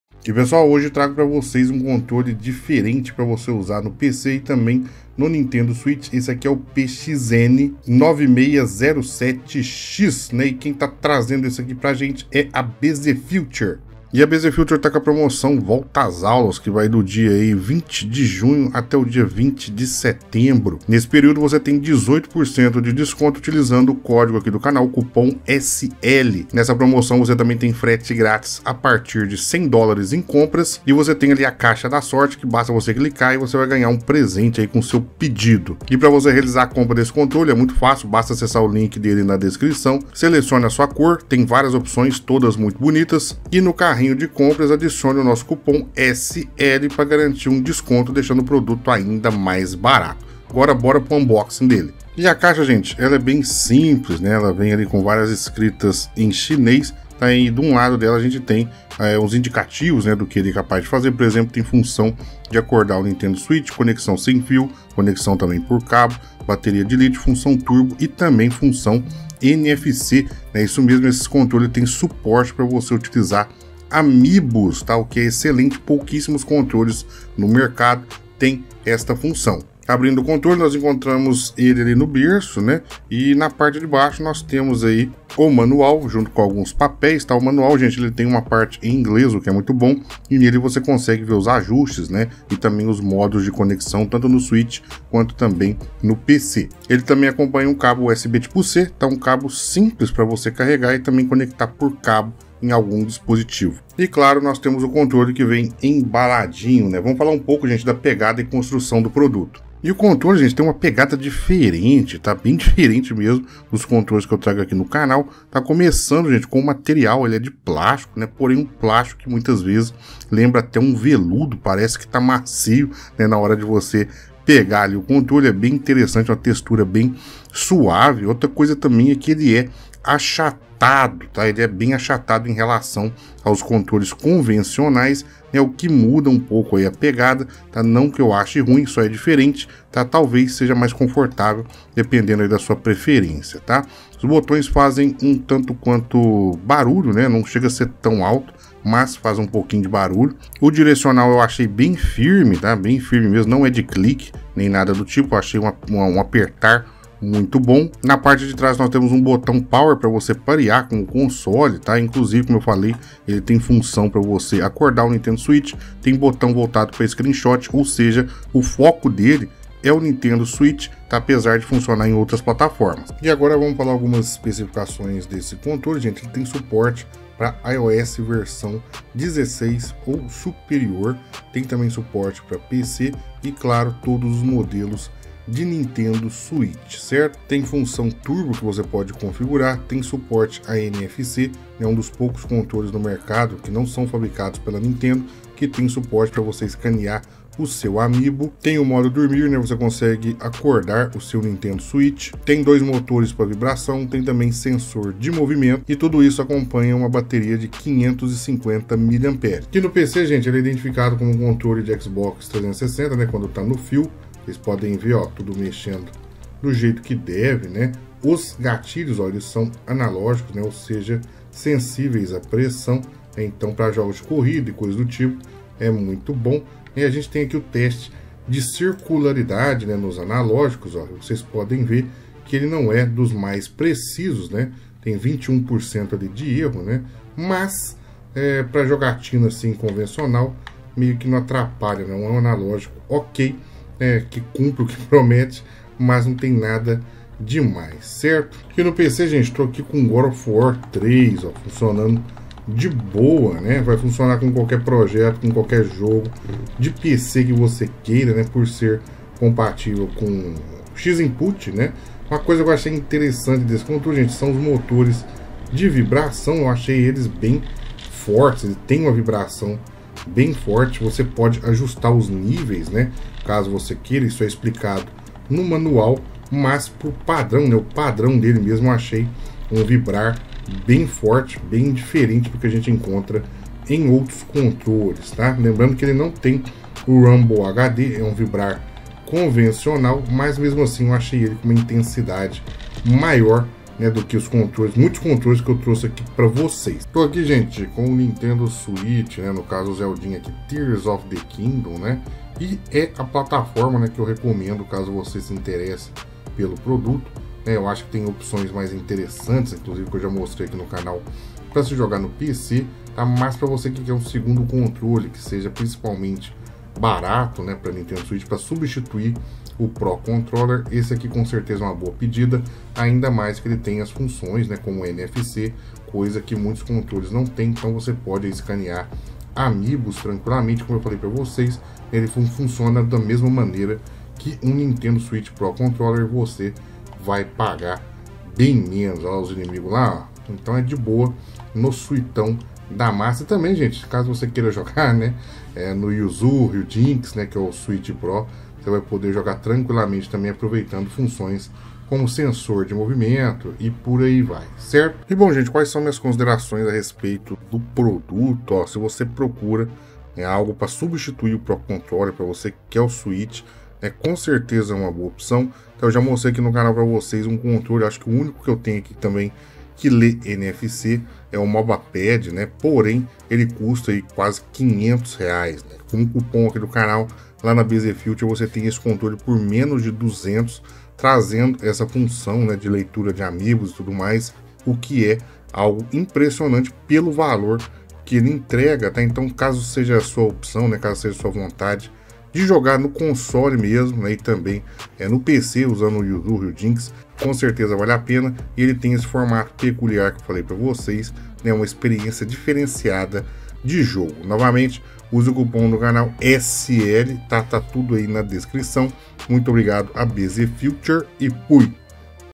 PXN 9607X é o Pro Controller MAIS BARATO do mercado! BZFUTURE
PXN 9607X é um controle barato que vem pra substituir o Pro Controller e tem até mesmo funções como leitura de Amiibos. Uma ótima alternativa para jogar no seu Nintendo Switch, com as funções dos controles oficiais da Nintendo. Veja uma análise completa!